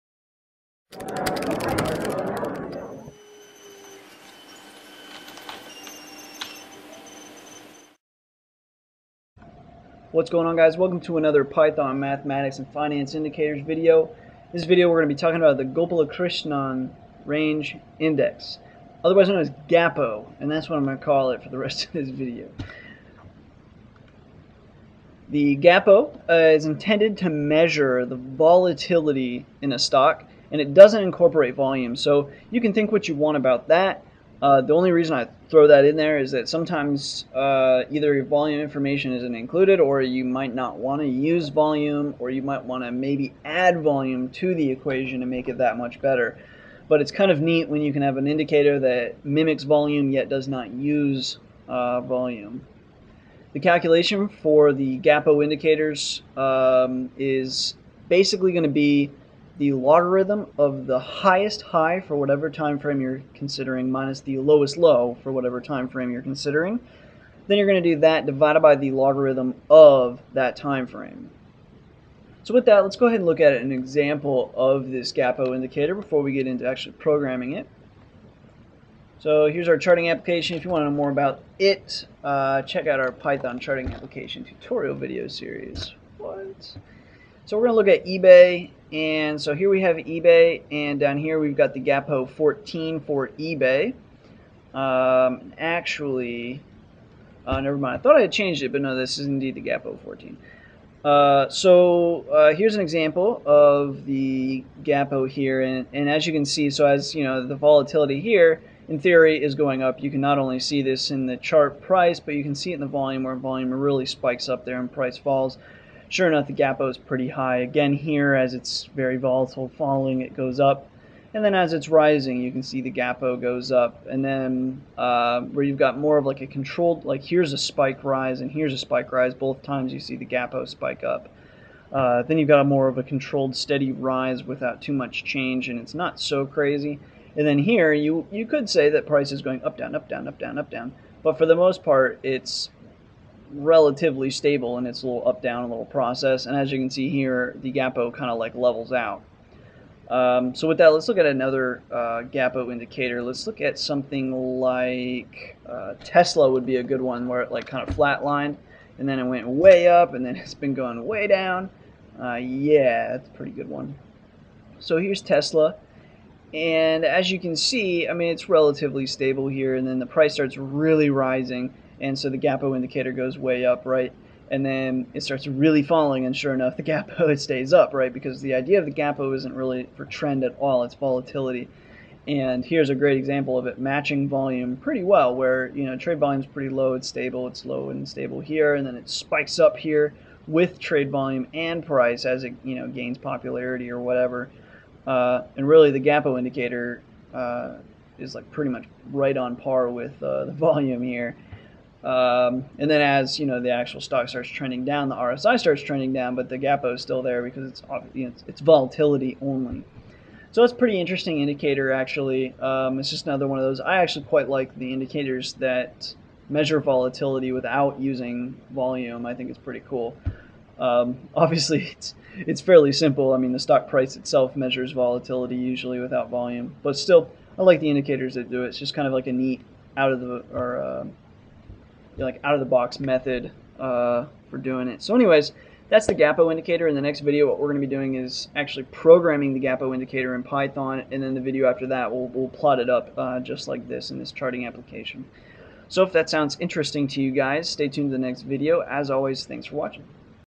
Python Finance and Math Tutorials: Gopalakrishnan Range Index (GAPO)
This tutorial video introduces you to the Gopalakrishnan Range Index (GAPO) indicator.

The Gopalakrishnan Range Index (GAPO) attempts to measure volatility in a stock.

The purpose of this series is to teach mathematics within python. To do this, we will be working with a bunch of the more popular stock indicators used with technical analysis.

With most of the indicators, we will first discuss them, their purpose, then teach how to program them into python, then actually display them on a chart.

Sentdex.com
Facebook.com/sentdex
Twitter.com/sentdex